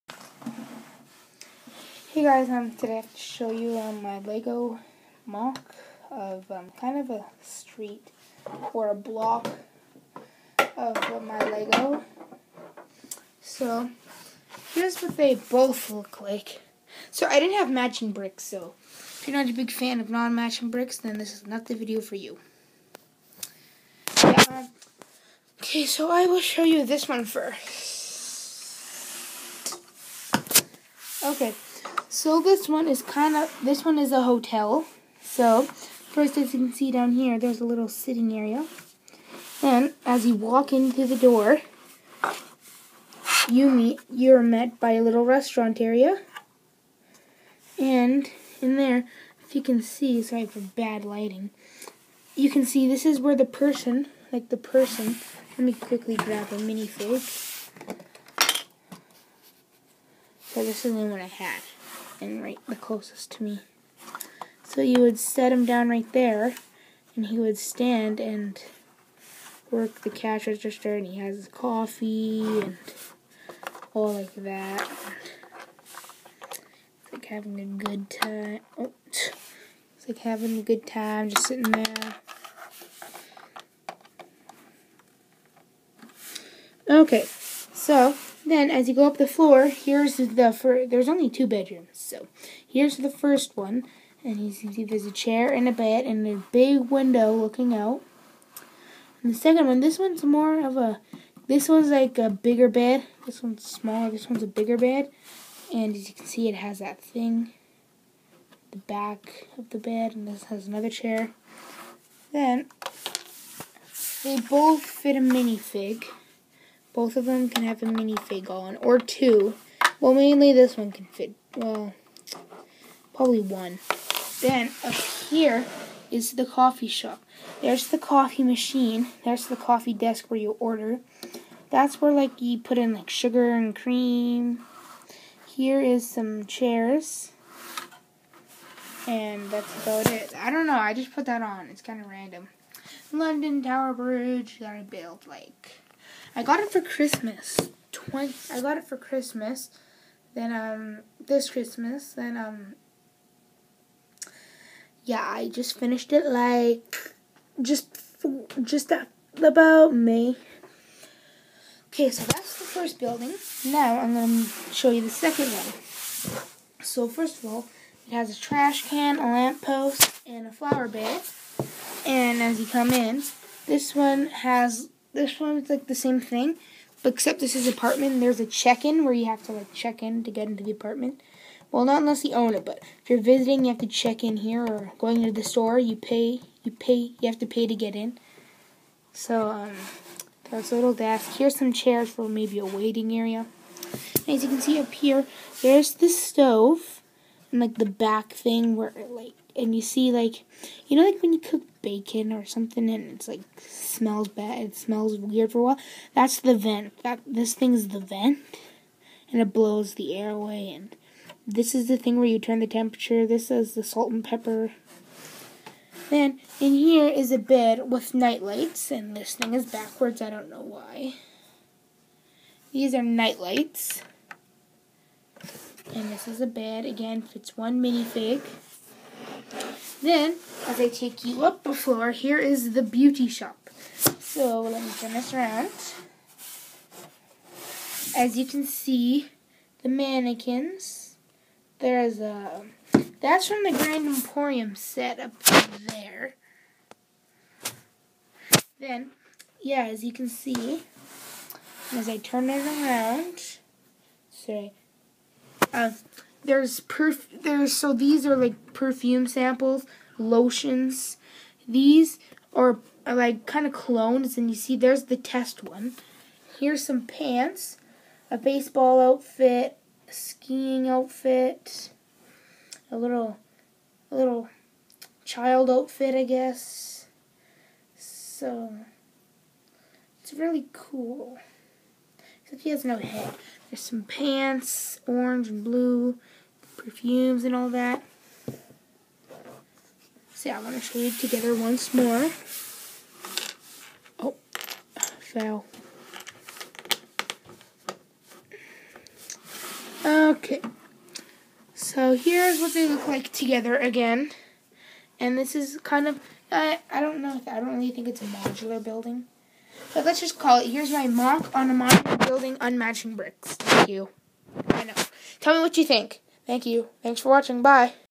lego modular moc please don't laugh.
this is a modular i built all by myself it's a little sketchy but I didn't have matching bricks hope you guys enjoy.